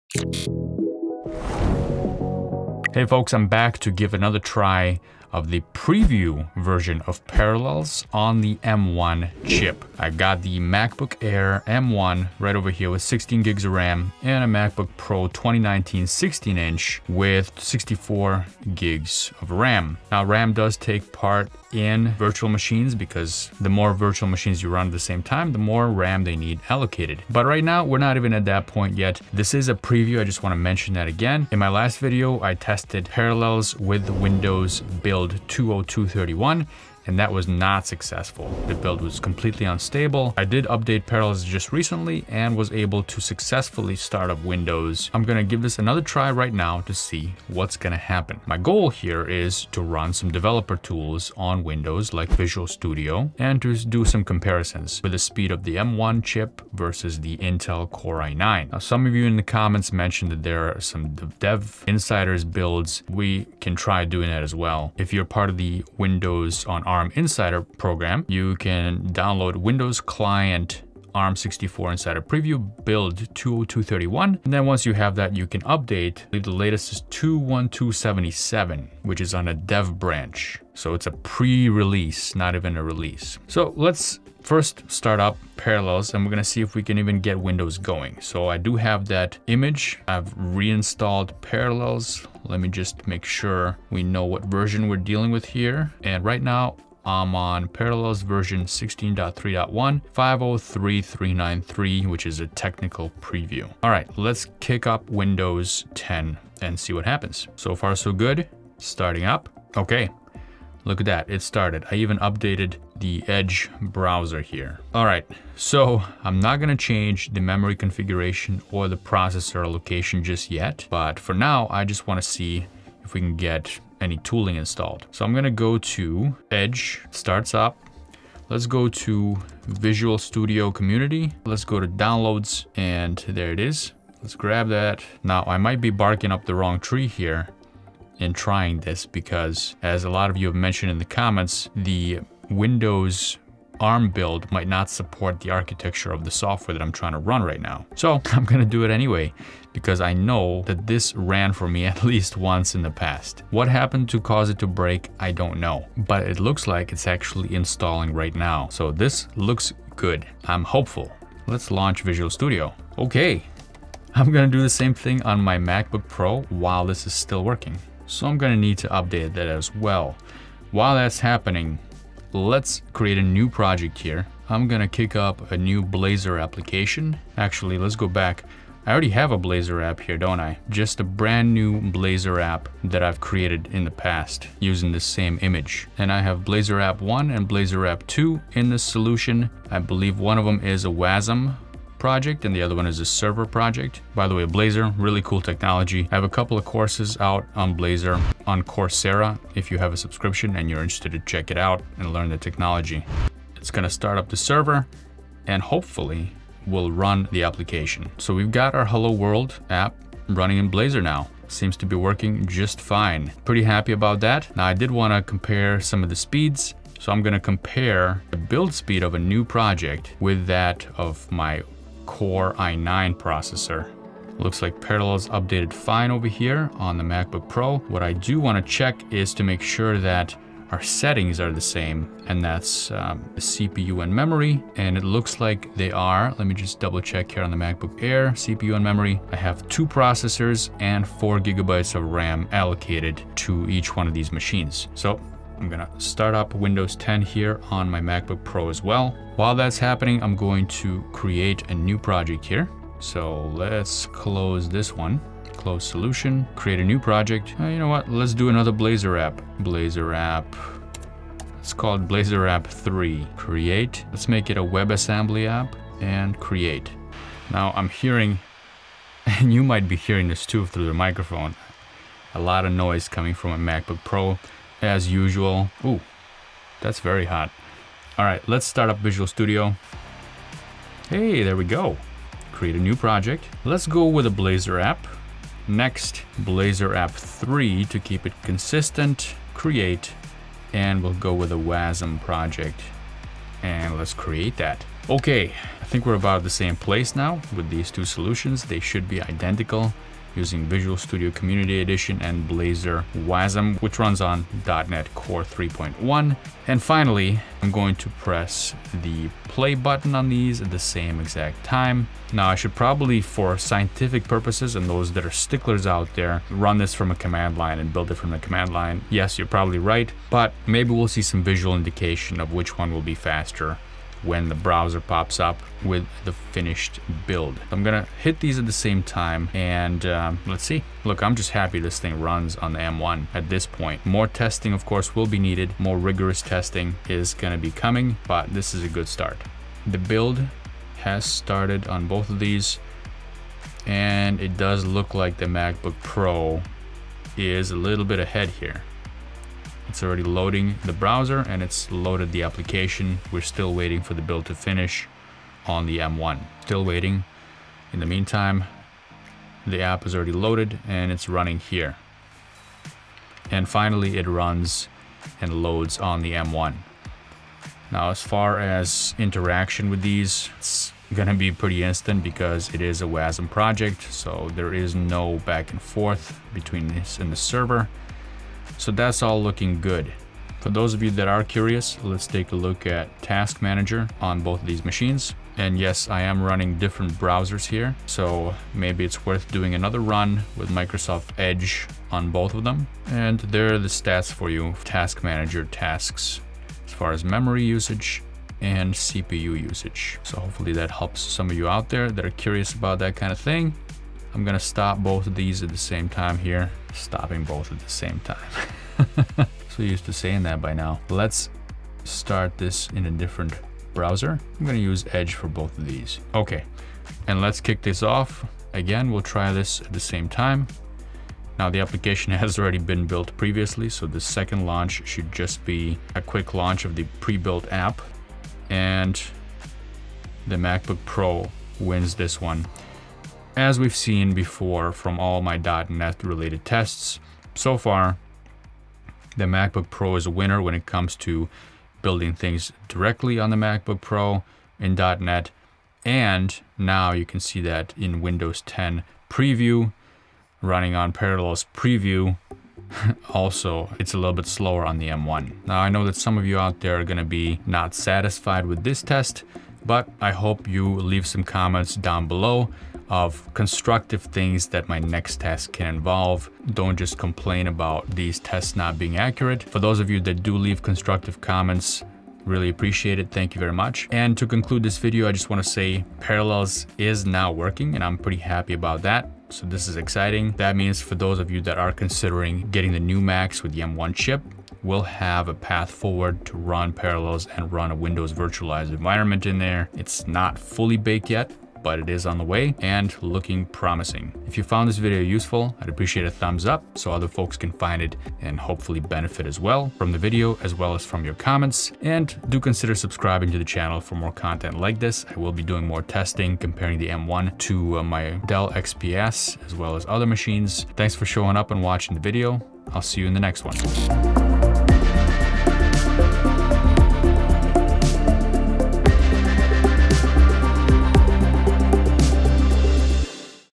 M1 MacBook with Parallels Running Visual Studio Projects
There is a new build of Parallels preview out for the ARM architecture that runs on the M1 MacBooks. I got it installed and running Windows while keeping my fingers crossed for any explosions like last time (VIDEO The goal is building a project in Visual Studio. And as is it becoming tradition on this channel to not only test the technologies, but also to pin it against my Intel Core i9 MacBook Pro.

🏫 FREE COURSES